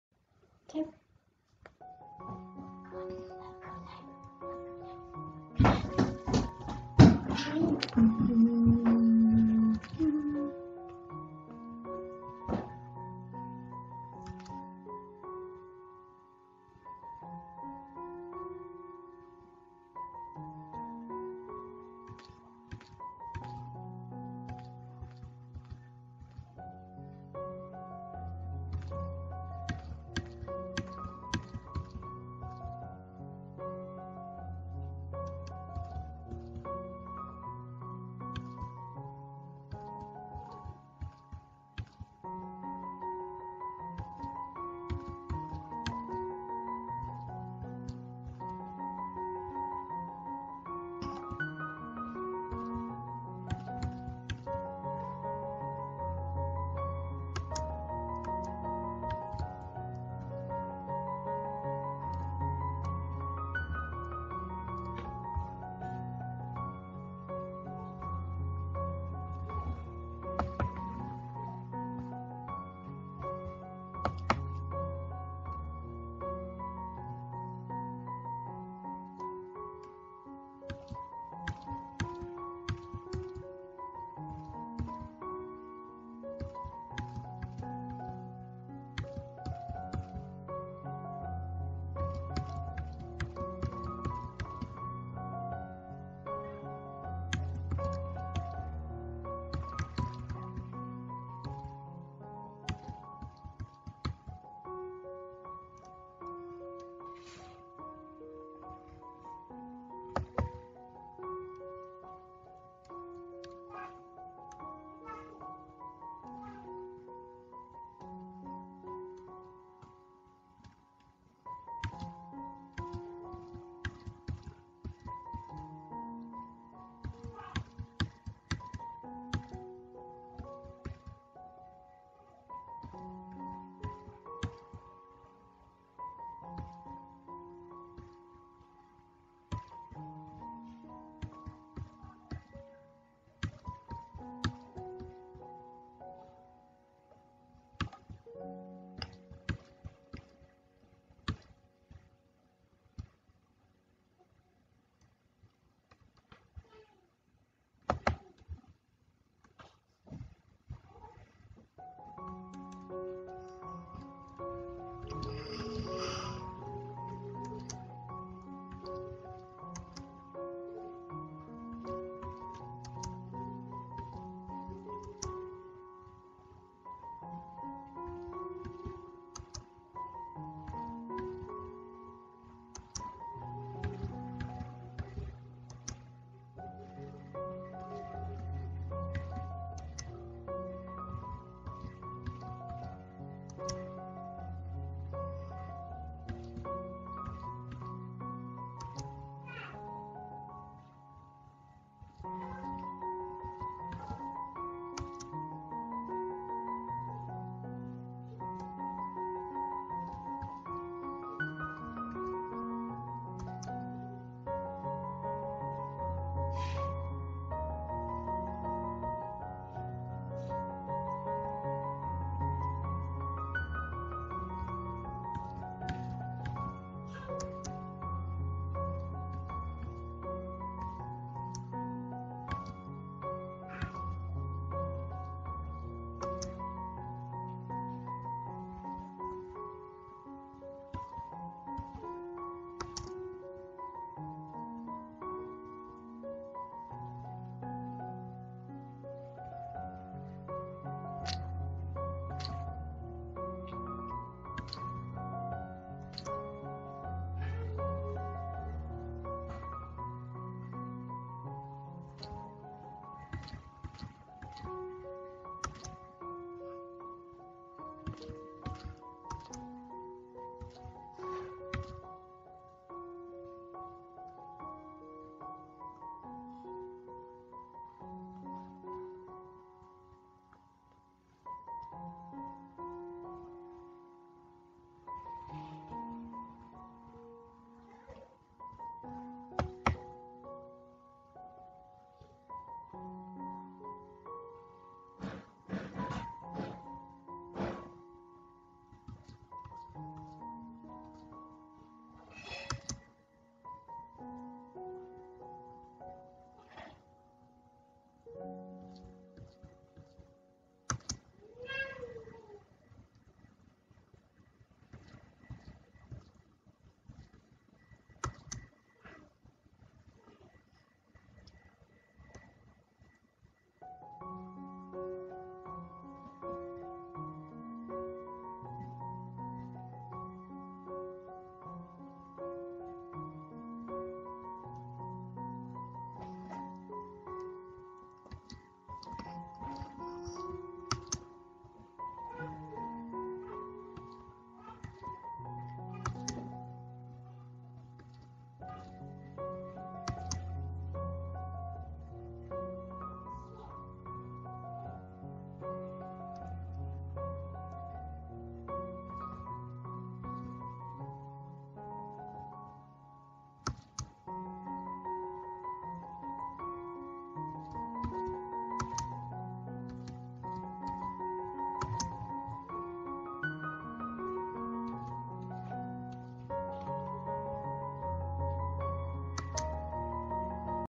Roxana's Home School Work "Typing Lesson 1"
Roxana is learning to type as part of here home school work.

These video's are just uploaded for her to look back on in the future and see how much she's improved as time goes by :)